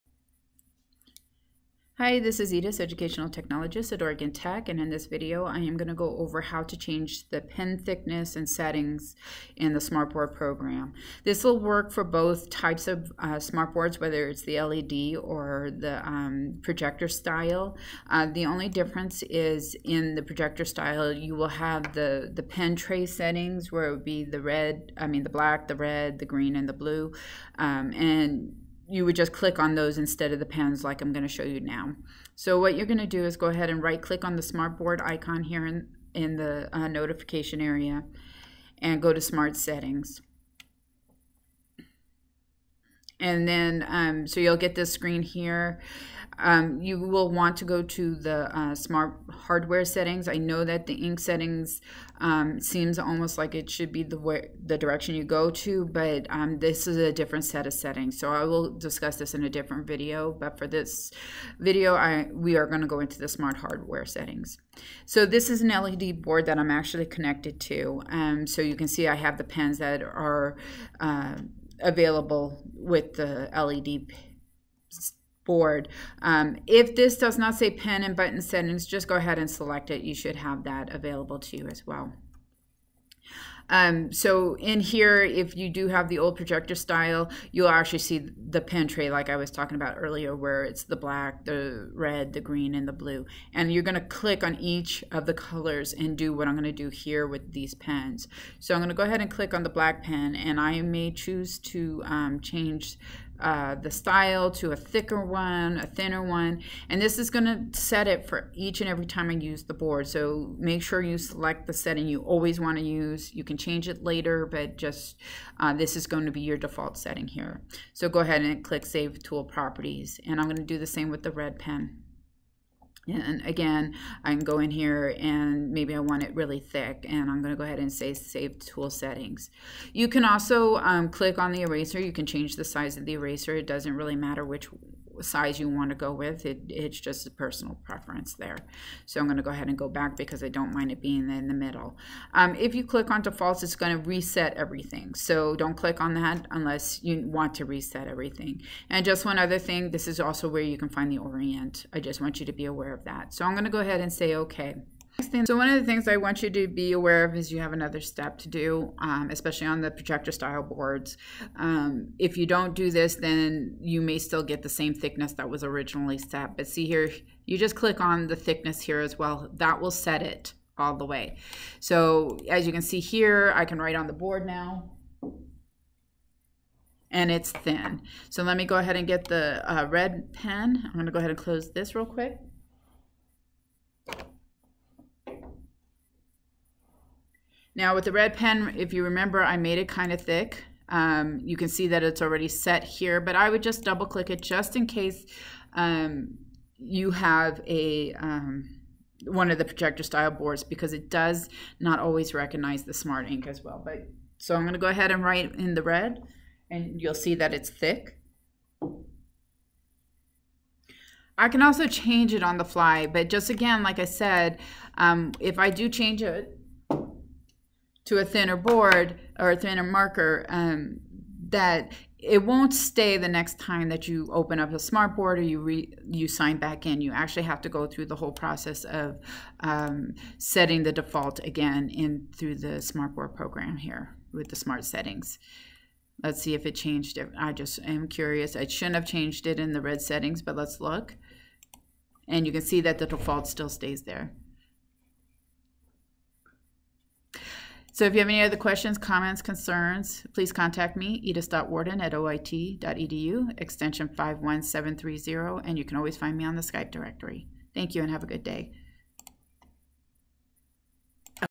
Smartboard Pens default settings
How to change the default pen settings for the Smartboards